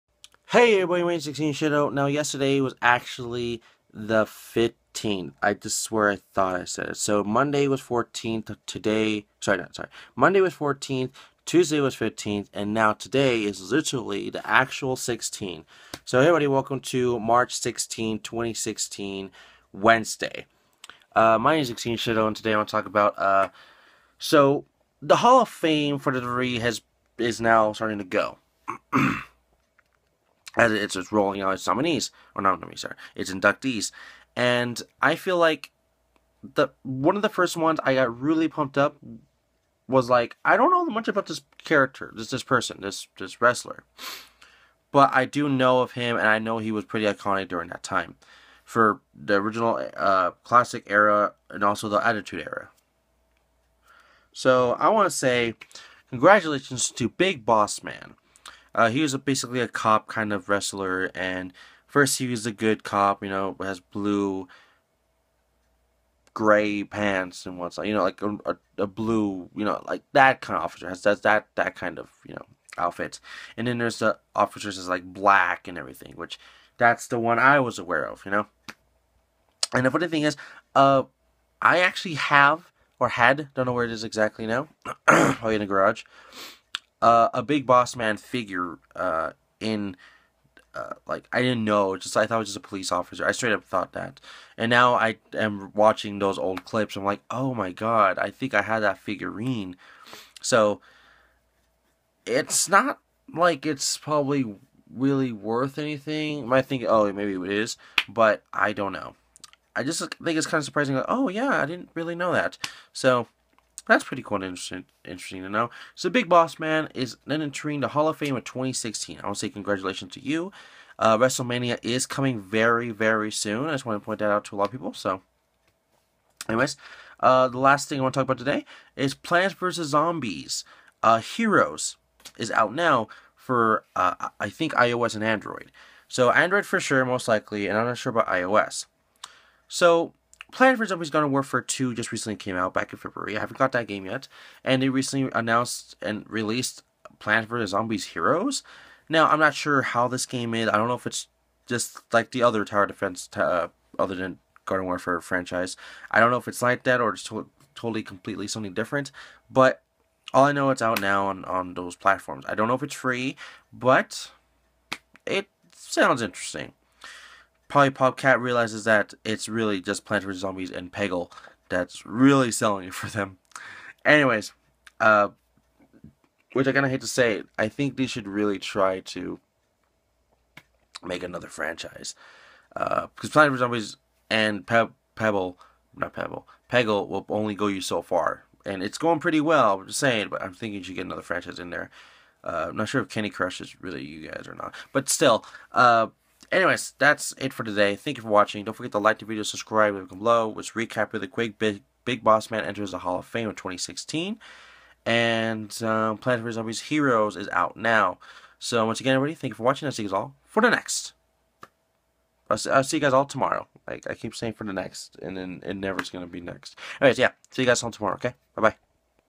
Plants vs Zombies Heroes Out Now & Hall of Fame Big Boss Man (3/17/16) Thursday
Another New Plants vs Zombies Game? also Big Boss Man in Hall of Fame.